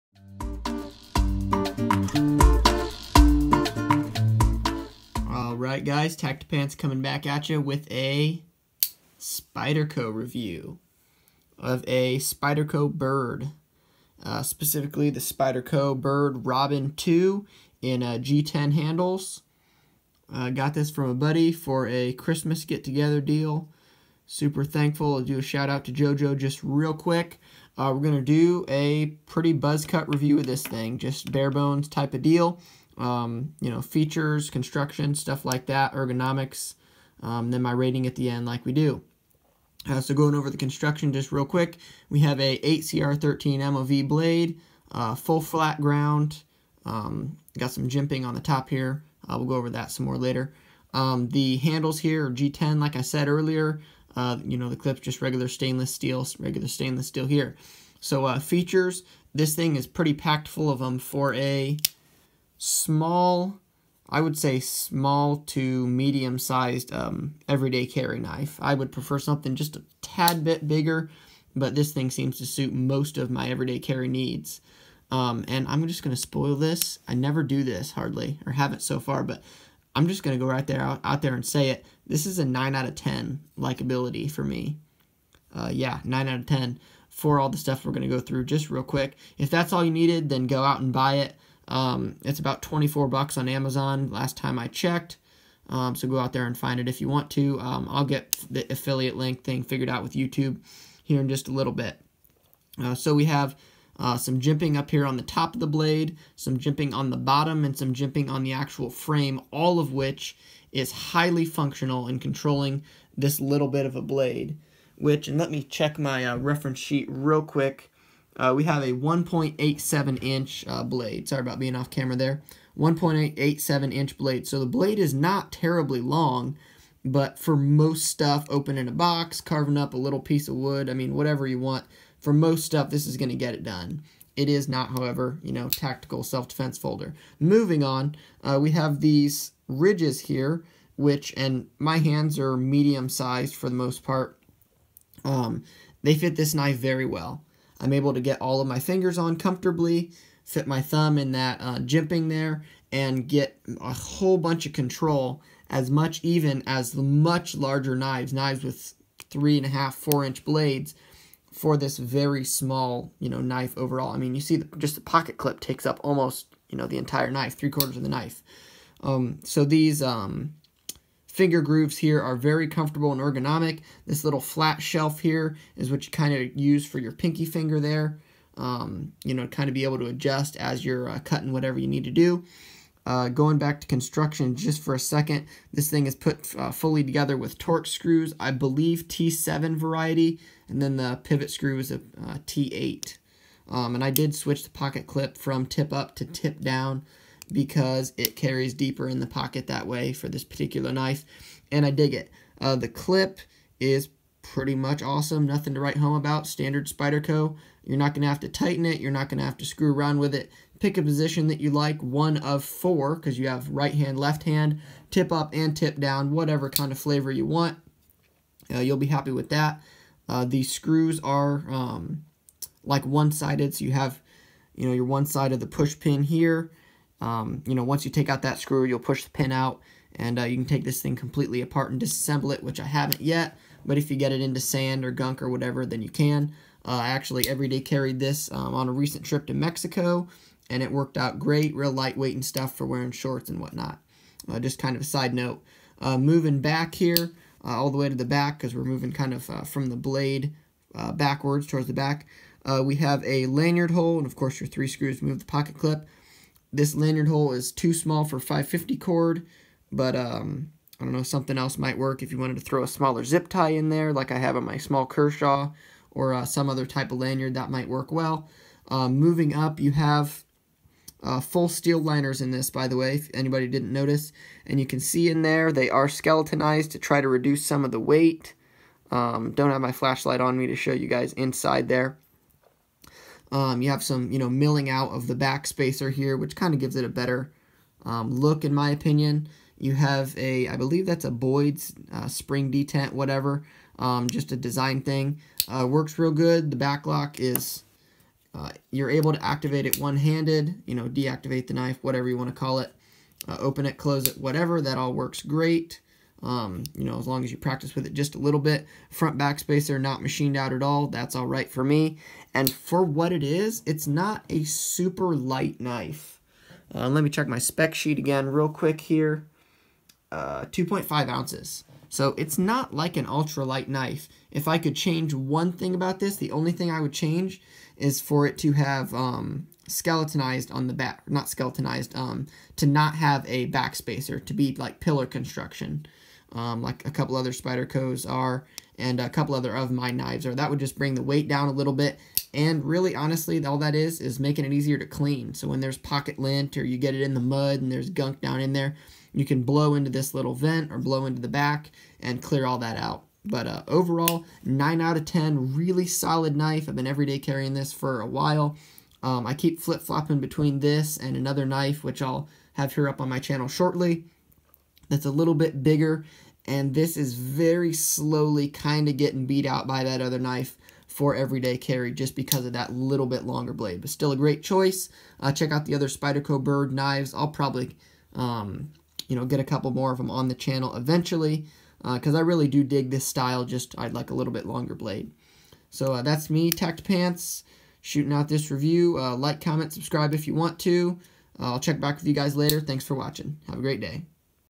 The Spyderco Byrd Robin 2, coconut carver!
The Spyderco Byrd Robin 2 is quite a mouthful but in the pocket or in the hand it shows that a name isn’t everything. The Robin 2 in G10 handles weighs 2.5 oz and has a bank vault solid lockup. For such a small knife I find that it often punches above its blade length in everyday tasks that I would normally favor a longer blade for. All in all, I rate the Robin 2 at a 9/10 based on my experience with it over the past 4+ months. -Tactipants